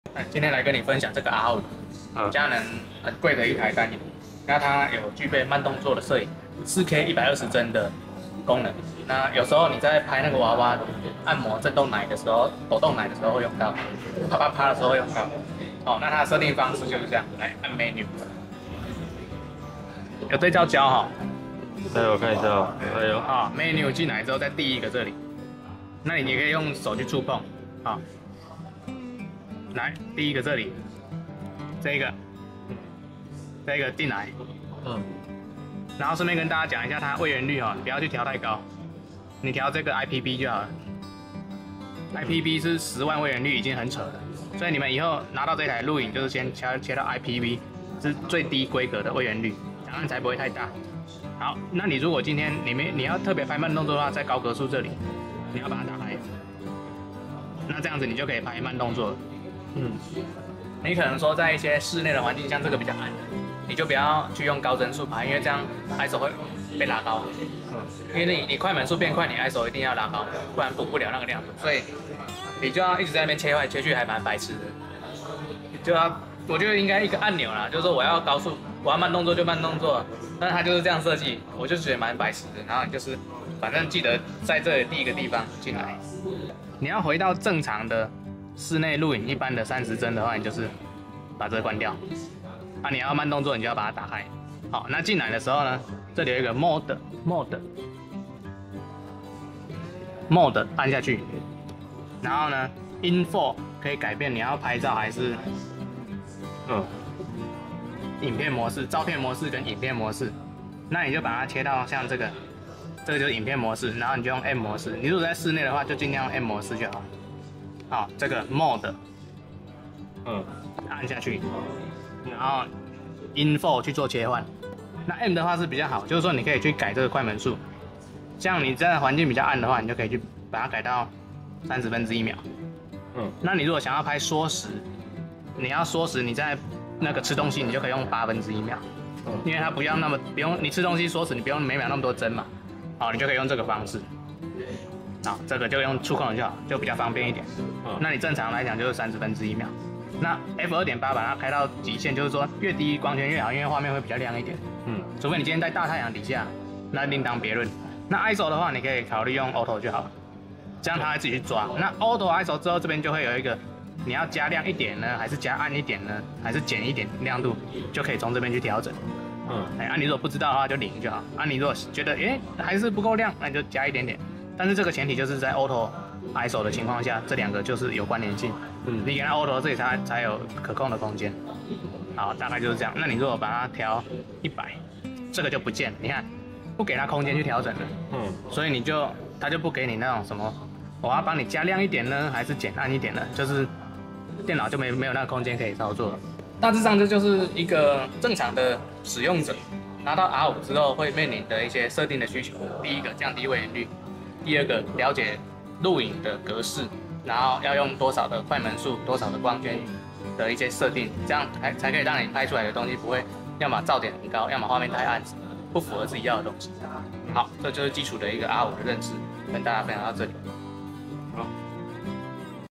CANON R5 拿到第一件事情 如何開啟慢動作錄影 降低位元率 避免空間浪費品質過剩
CANON R5